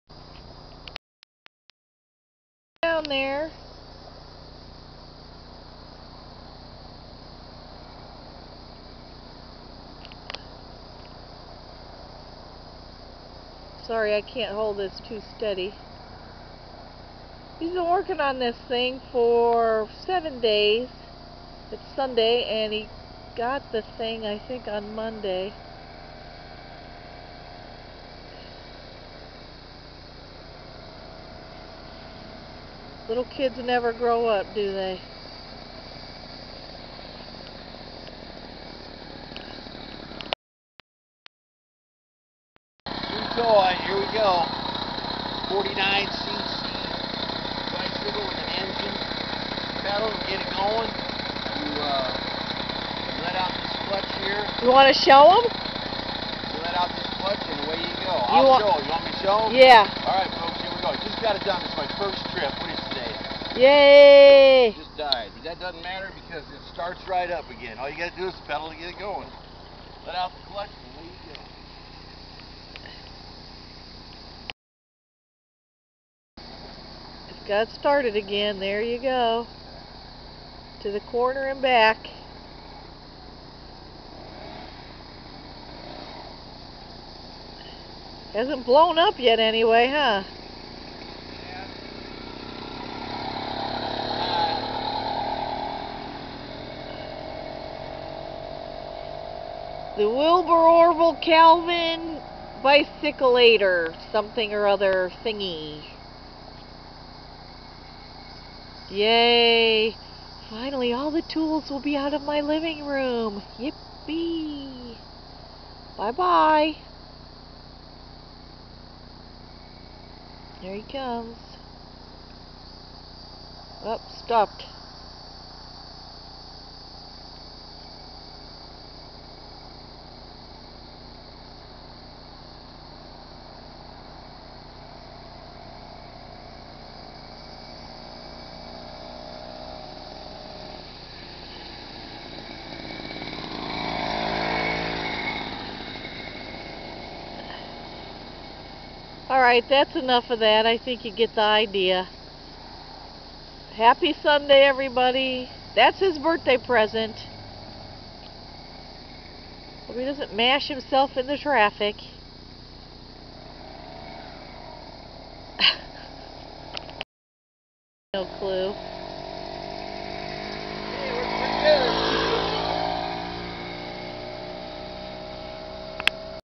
motorbike 010.avi
riding on the motorbike finally...what a great birthday present!